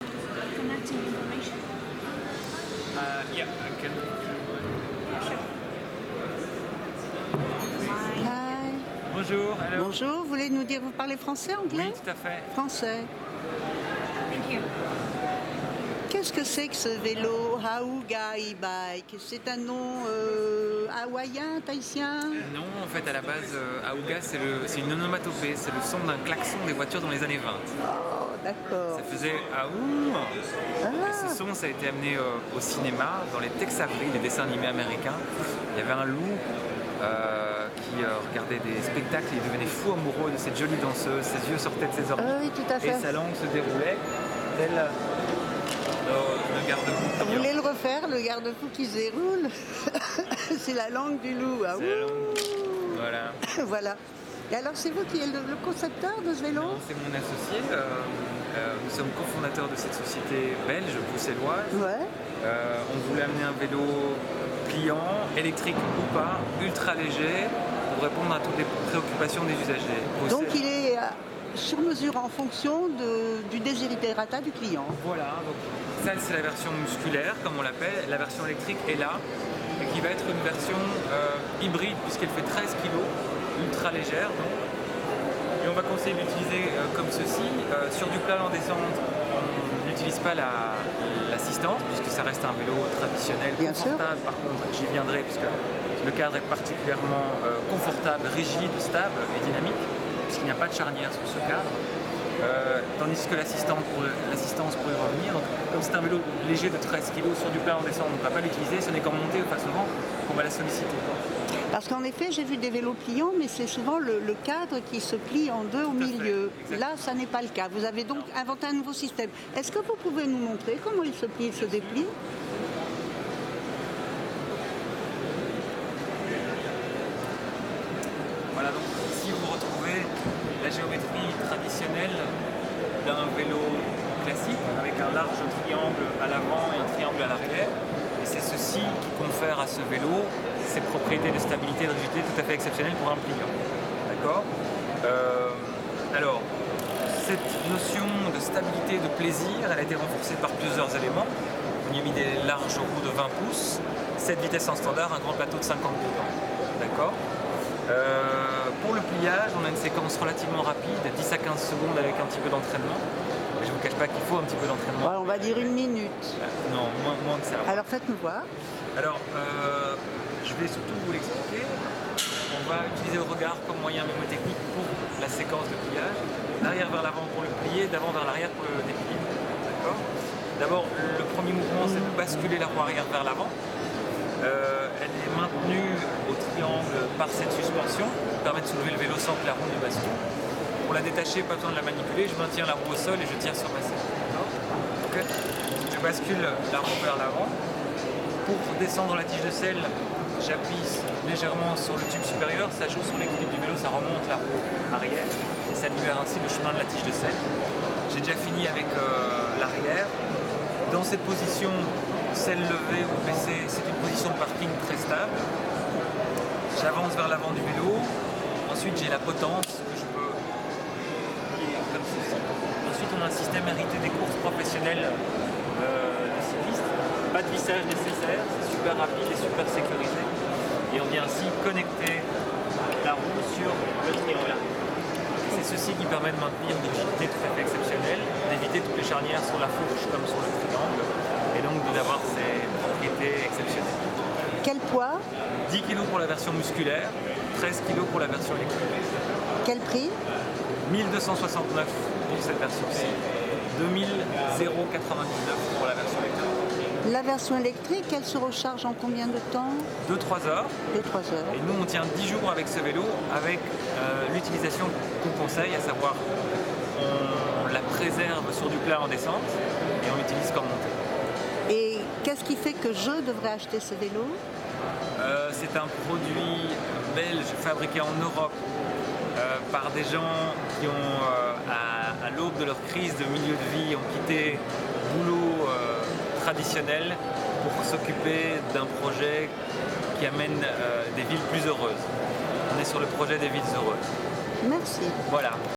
LikeBike Monaco 2016. Ahooga Ebike : la référence en matière de vélo à assistance électrique urbain
LikeBike Monaco  2016. Ahooga Ebike : la référence en matière de vélo à assistance électrique urbain, pliants, électriques ou pas, ultra léger